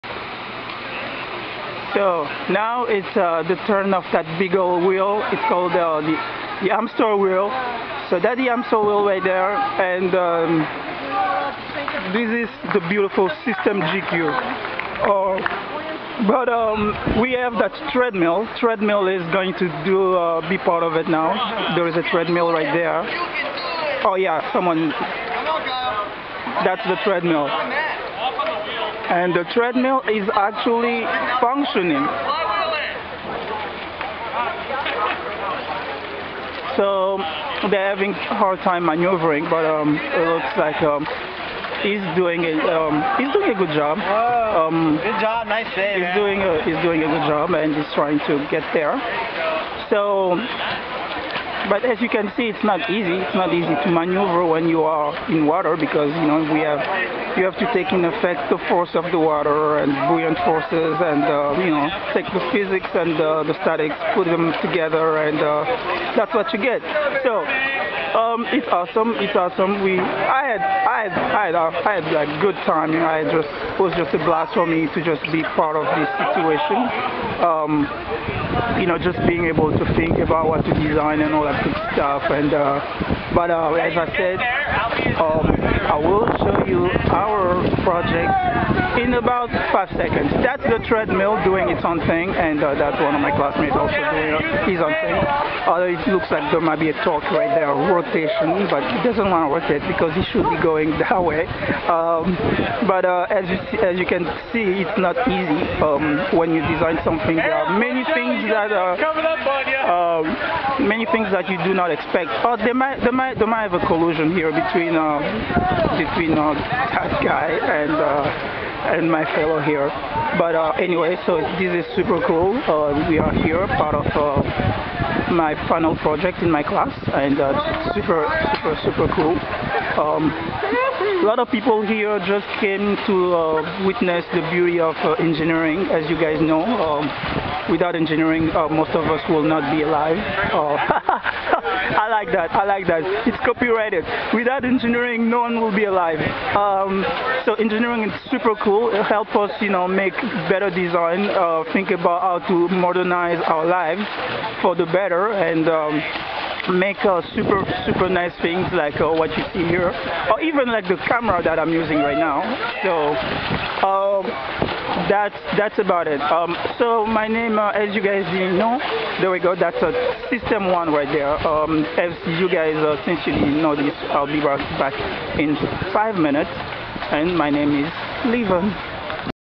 Engineering Works! Walk on H20?
Can be done! Using imagination, creativity, hard work combine with knowledge of some physics and statics of course! ... can one really walk on water?